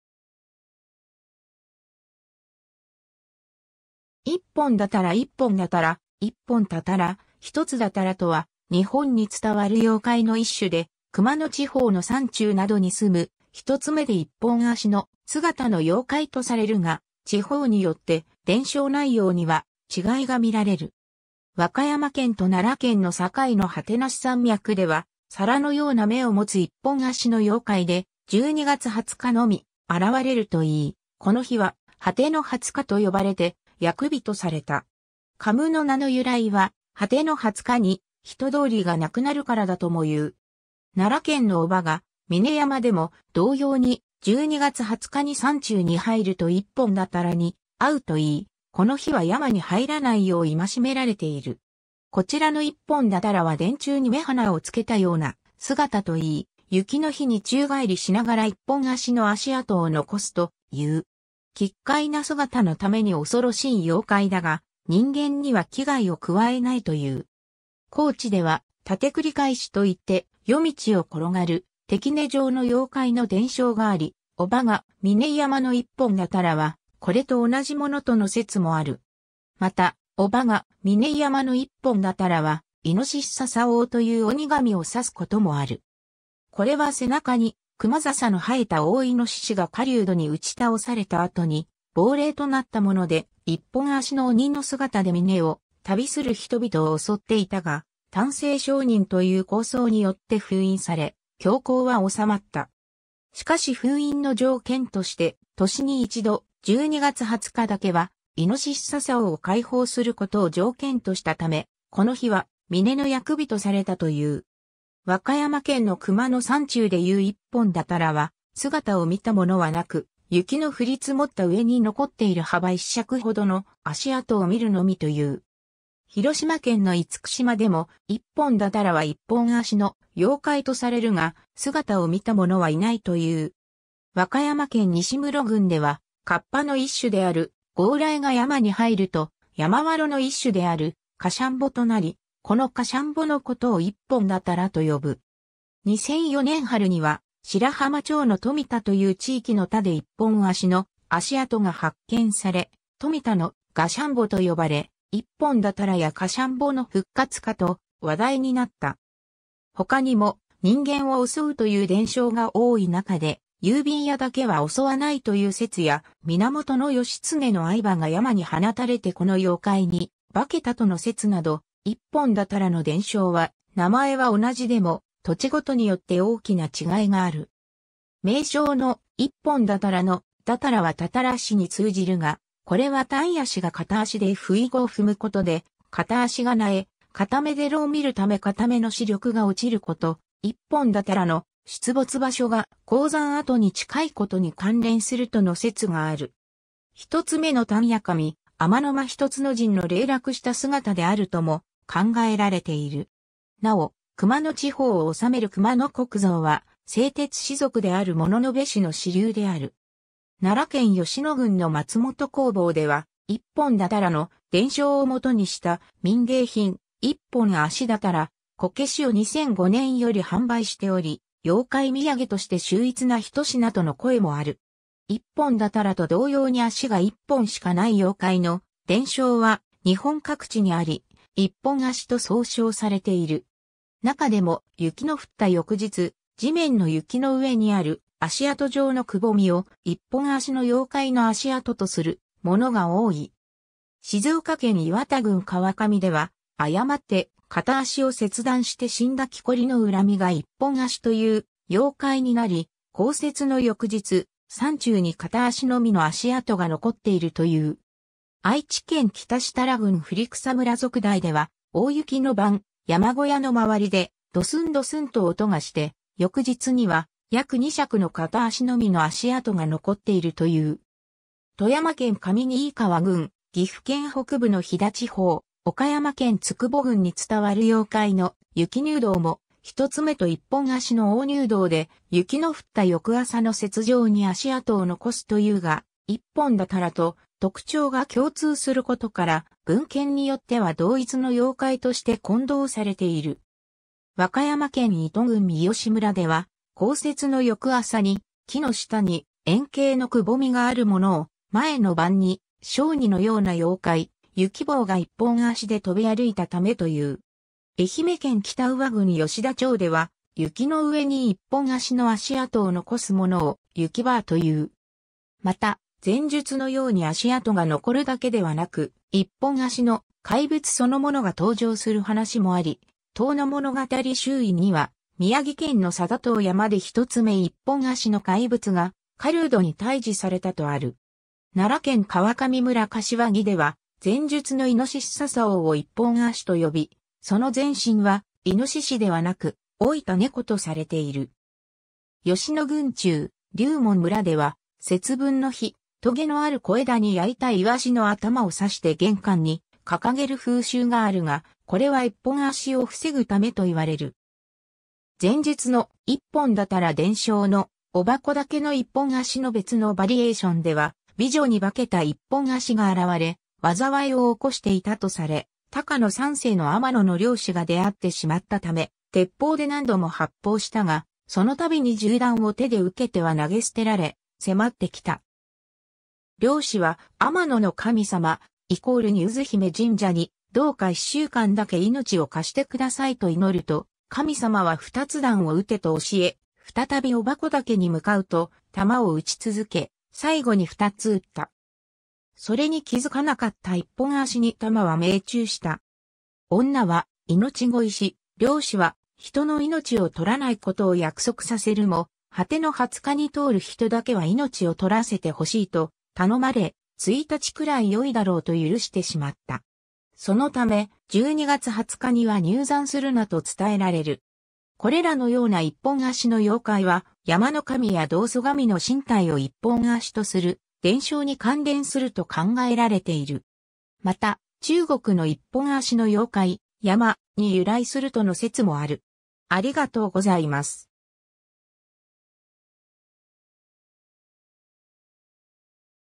一本だたら
一本だたら（模型） 一本だたら、一本踏鞴（いっぽんだたら）、一つだたら（ひとつだたら）とは、日本に伝わる妖怪の一種で、熊野地方の山中などに棲む、一つ目で一本足の姿の妖怪とされるが、地方によって伝承内容には違いが見られる。
 和歌山県と奈良県の境の果無山脈では、皿のような目を持つ一本足の妖怪で、12月20日のみ現れるといい、この日は「果ての二十日」と呼ばれて厄日とされた。
果無の名の由来は「果ての二十日」に人通りが無くなるからだともいう。
 奈良県の伯母ヶ峰山でも同様に、12月20日に山中に入ると一本だたらに遭うといい、この日は山に入らないよう戒められている。
こちらの一本だたらは電柱に目鼻をつけたような姿といい、雪の日に宙返りしながら一本足の足跡を残すという。
奇怪な姿のために恐ろしい妖怪だが、人間には危害を加えないという。
高知では「タテクリカエシ」といって、夜道を転がる手杵状の妖怪の伝承があり、伯母ヶ峰山の一本だたらはこれと同じものとの説もある。
 また伯母ヶ峰山の一本だたらは、猪笹王（いのささおう）という鬼神を指すこともある。
これは背中に熊笹の生えた大イノシシが狩人に撃ち倒された後に亡霊となったもので、一本足の鬼の姿で峰を旅する人々を襲っていたが、丹誠上人という高僧によって封印され、凶行はおさまった。
しかし封印の条件として年に一度、12月20日だけは猪笹王を解放することを条件としたため、この日は峰の厄日とされたという。
 和歌山県の熊野山中でいう一本だたらは、姿を見た者はなく、雪の降り積もった上に残っている幅1尺ほどの足跡を見るのみという。
広島県の厳島でも、一本だたらは一本足の妖怪とされるが、姿を見た者はいないという。
 和歌山県西牟婁郡では、カッパの一種である「ゴーライ」が山に入ると、山童の一種である「カシャンボ」となり、このカシャンボのことを一本だたらと呼ぶ。
2004年春には、白浜町の富田という地域の田で1本足の足跡が発見され、「富田のがしゃんぼ」と呼ばれ、一本だたらやカシャンボの復活かと話題になった。
 他にも、人間を襲うという伝承が多い中で郵便屋だけは襲わないという説や、源義経の愛馬が山に放たれてこの妖怪に化けたとの説など、一本だたらの伝承は名前は同じでも、土地ごとによって大きな違いがある。
 名称の「一本だたら」の「だたら」はタタラ師（鍛冶師）に通じるが、これは鍛冶師が片足で鞴を踏むことで片脚が萎え、片目で炉を見るため片目の視力が落ちること、一本だたらの出没場所が鉱山跡に近いことに関連するとの説がある（隻眼#神話・伝説の中の隻眼を参照）。
一つ目の鍛冶神、天目一箇神（あめのまひとつのかみ）の零落した姿であるとも考えられている。
なお熊野地方を治める熊野国造は製鉄氏族である物部氏の支流である。
 奈良県吉野郡の松本工房では、一本だたらの伝承をもとにした民芸品「一本足だたらこけし」を2005年より販売しており、妖怪土産として秀逸な一品との声もある。
 一本だたらと同様に足が1本しかない妖怪の伝承は日本各地にあり、一本足（いっぽんあし）と総称されている。
中でも、雪の降った翌日、地面の雪の上にある足跡状の窪みを、一本足の妖怪の足跡とするものが多い。
 静岡県磐田郡（現・浜松市天竜区）川上では、「誤って片足を切断して死んだ木こりの怨みが一本足という妖怪になり、降雪の翌日、山中に片足のみの足跡が残っている」という。
 愛知県北設楽郡振草村粟代（現・設楽町）では、「大雪の晩、山小屋の周りで、ドスンドスンと音がして、翌日には約2尺（約60センチメートル）の片足のみの足跡が残っている」という。
 富山県上新川郡（現・富山市）、岐阜県北部の飛騨地方、岡山県都窪郡に伝わる妖怪の「雪入道」（ゆきにゅうどう）も、一つ目と一本足の大入道で、「雪の降った翌朝の雪上に足跡を残す」というが、一本だたらと特徴が共通することから、文献によっては同一の妖怪として混同されている。
 和歌山県伊都郡三好村（現・かつらぎ町）では、降雪の翌朝に木の下に円形の窪みがあるものを、前の晩に小児のような妖怪「雪坊（ゆきんぼ）」が一本足で飛び歩いたためという。
 愛媛県北宇和郡吉田町（現・松野町）では、雪の上に一本足の足跡を残すものを「雪婆（ゆきんば）」という。
 また、前述のように足跡が残るだけではなく、一本足の怪物そのものが登場する話もあり、『遠野物語拾遺』には、宮城県の貞任山で一つ目一本足の怪物が、狩人に退治されたとある。
 奈良県川上村柏木では、前述の猪笹王を一...